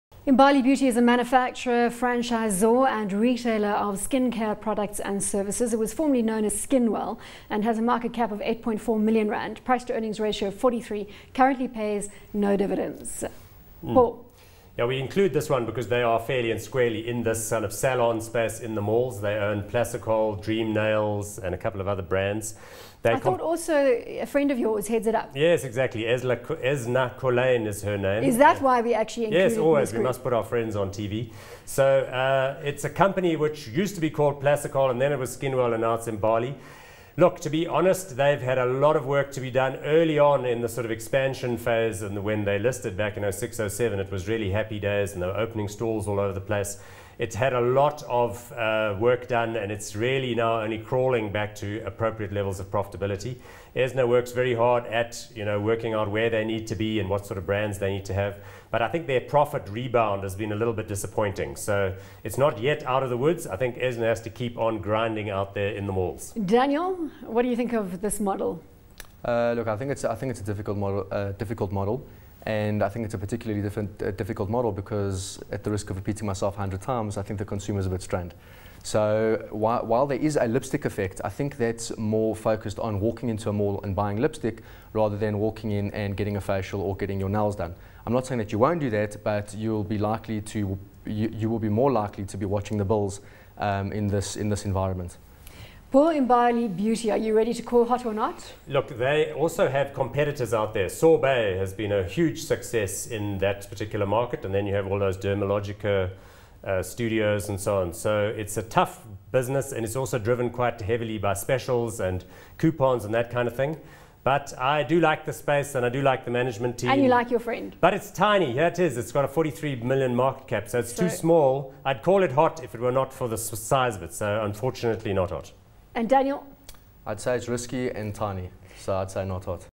Imbalie - Hot or Not
Imbalie is a manufacturer, franchisor and retailer of skincare products and services. It was formerly known as Skinwell, and has a market cap of R8.4 million and a price to earnings ratio of 43, but currently pays no dividends.